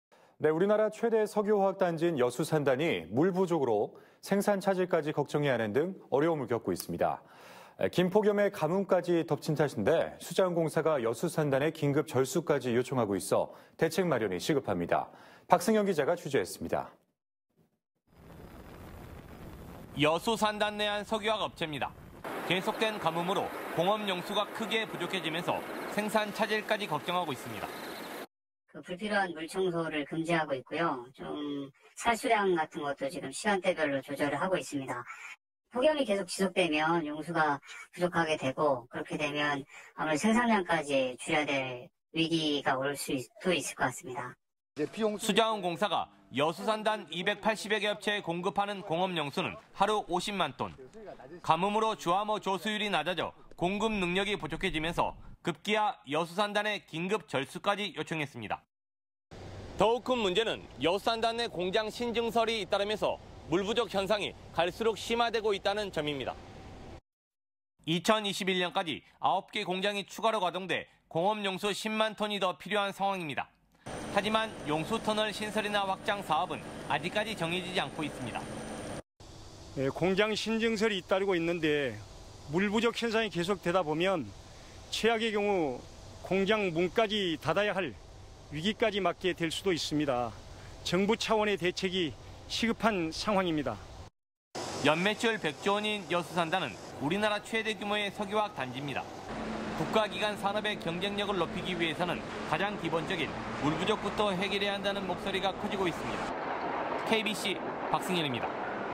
여수산단 물부족 심각..긴급 절수, 생산 차질 우려
우리나라 최대 석유화학단지인 여수산단이 물 부족으로 생산 차질까지 걱정해야 하는 등 어려움을 겪고 있습니다.

긴 폭염에 가뭄까지 덮친 탓인데, 수자원공사가 여수산단에 긴급절수까지 요청하고 있어 대책마련이 시급합니다.